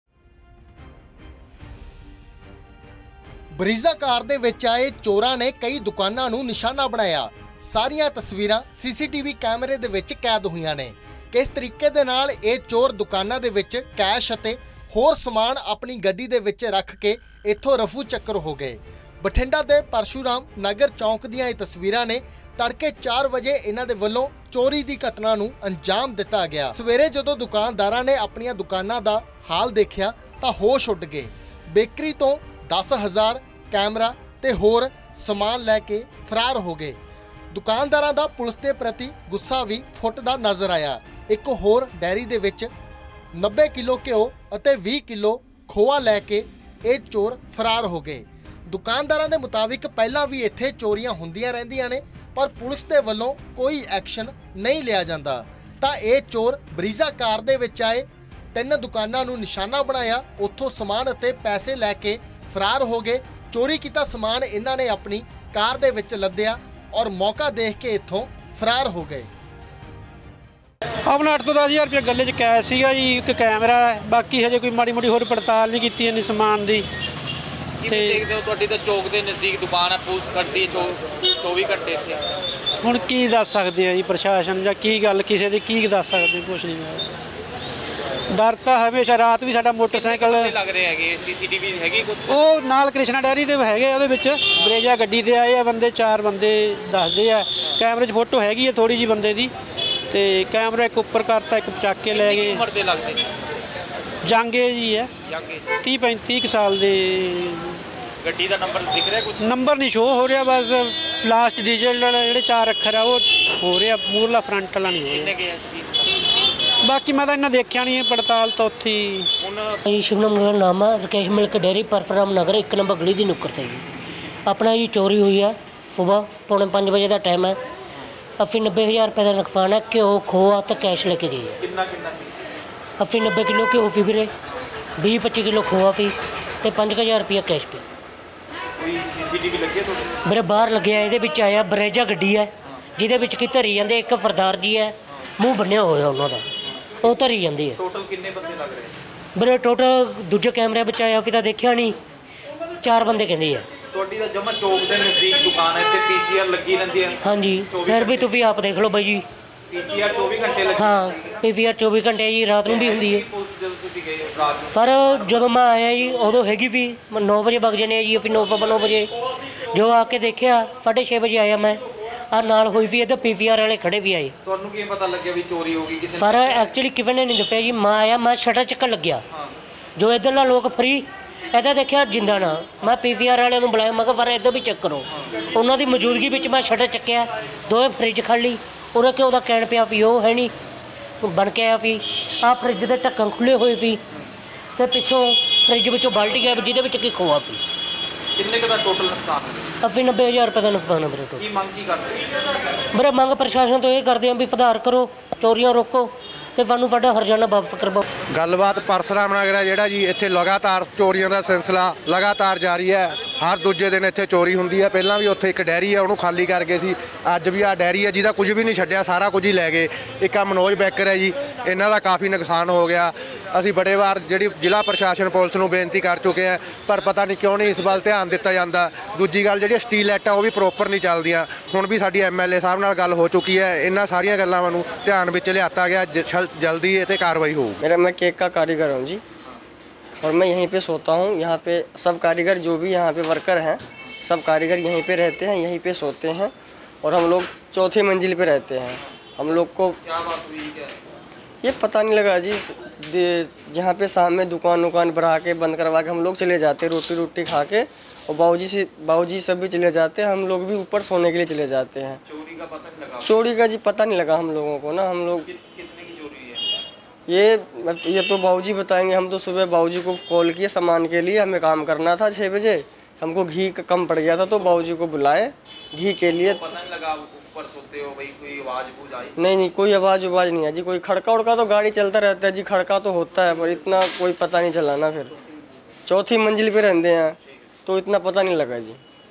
Bathinda CCTV Chori | ਬਰੈਜ਼ਾ ਗੱਡੀ 'ਚ ਆਏ ਚੋਰਾਂ ਨੇ ਕਈ ਦੁਕਾਨਾਂ ਕੀਤੀਆਂ ਖਾਲੀ! | Punjab News| N18V
Bathinda CCTV Chori | ਬਰੈਜ਼ਾ ਗੱਡੀ 'ਚ ਆਏ ਚੋਰਾਂ ਨੇ ਕਈ ਦੁਕਾਨਾਂ ਕੀਤੀਆਂ ਖਾਲੀ! | N18V